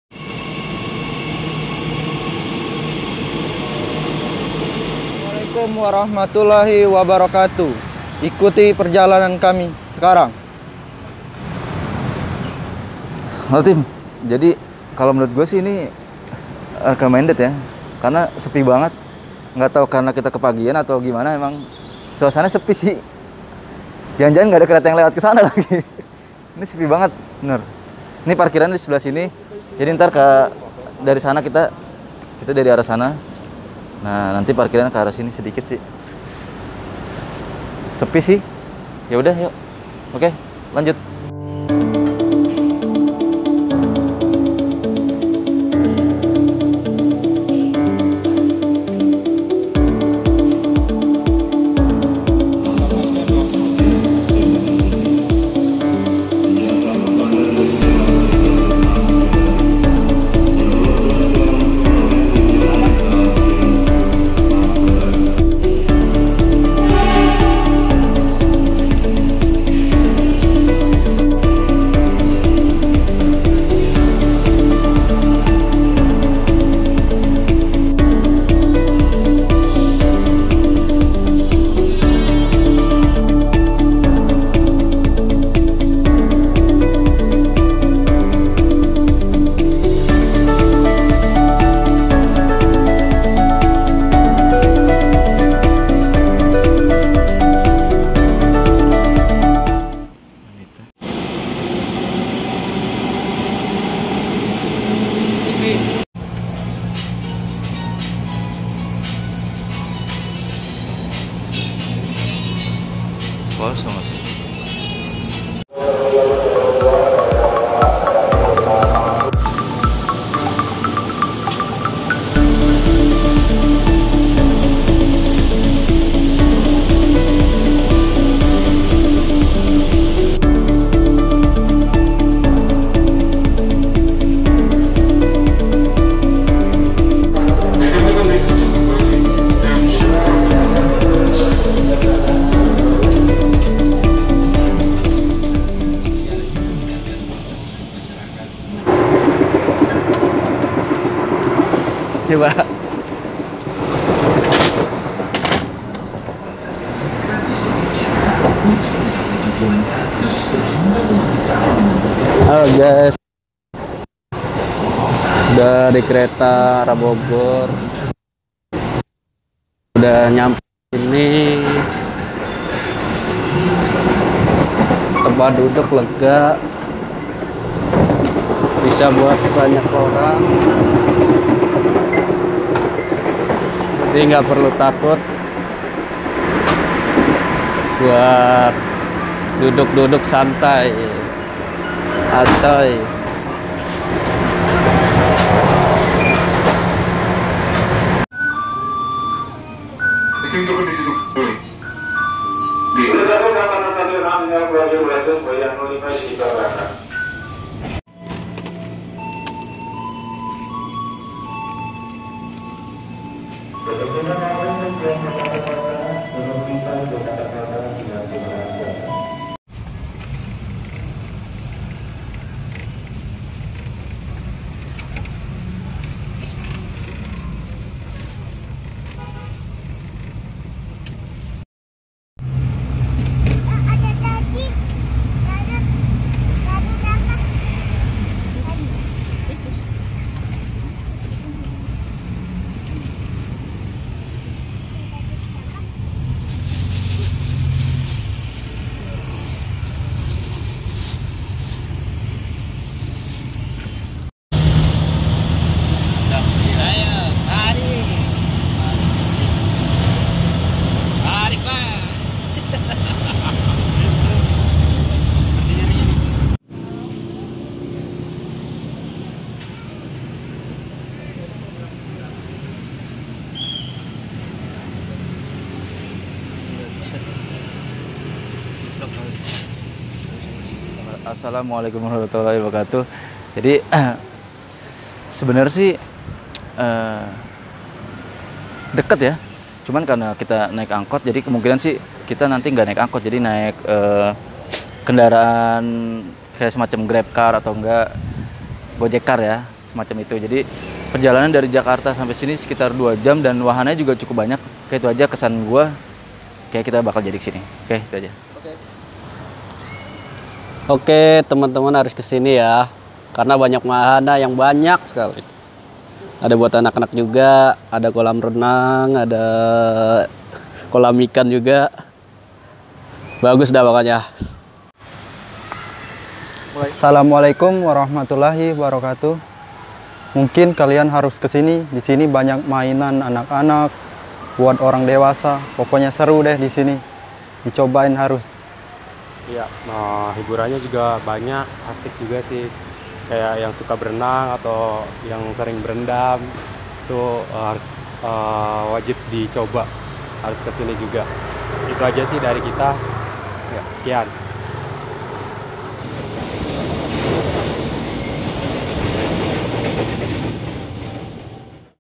Akses Mudah The Jungle || Jalur KERETA dan Angkot || Jakarta-Bogor || 50ribu udah PP
Video ini di mulai dari

Stasiun
BNI CITY - MANGGARAI - BOGOR

PP Rute sebaliknya Biaya 24ribu